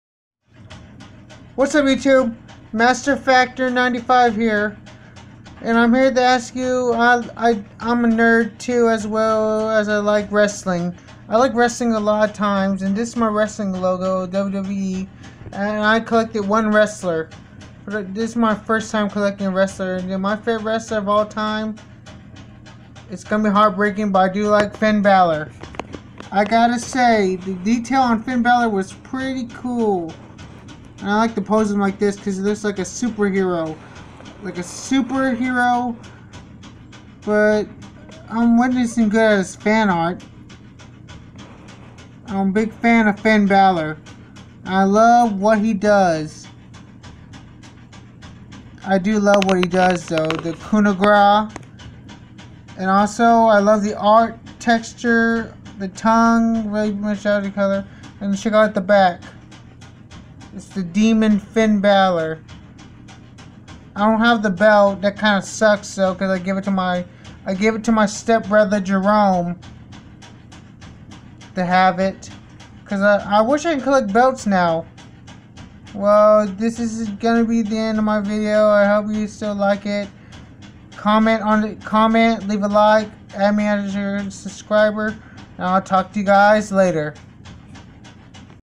WWE Figure Started Finn Balor
I'm starting to Collect WWE Figures this is my first figure
Of them all please leave a liked in this video..